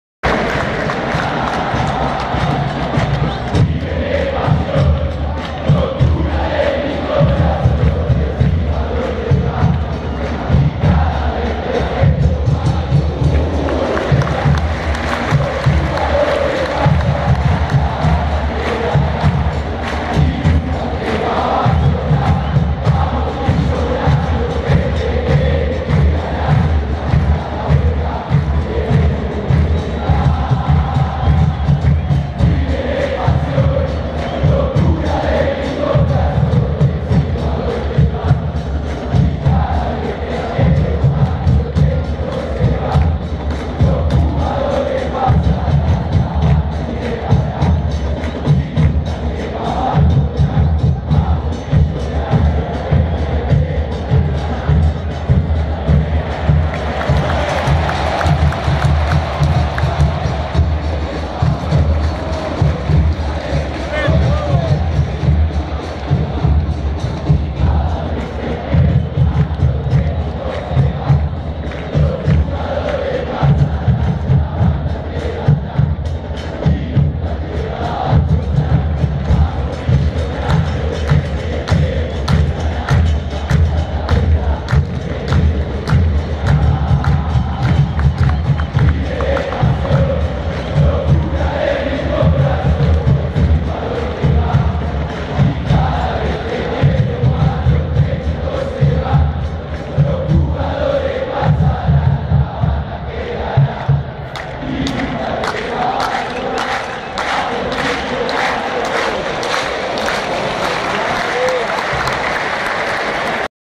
RIVER ES PASION - River Plate vs Libertad - Copa Sudamericana 2014
Copa Sudamericana 2014 - 8vos de Final - River Plate vs Libertad

•Partido tranquilo para el River del Muñeco Gallardo. Da gusto ver a este equipo jugar. Nos vemos en 4tos!